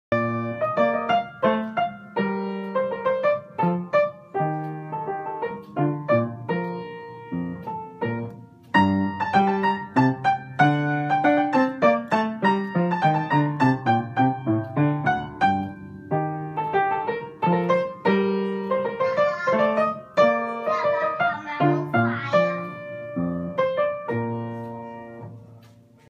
Awe-Inspiring 7-Yr-Old Piano Prodigy
In this episode of PRODIGIES, THNKR introduces you to the phenomenally talented Gavin George.

At 9 years old, Gavin deftly plays some of the most difficult piano pieces ever composed and performs at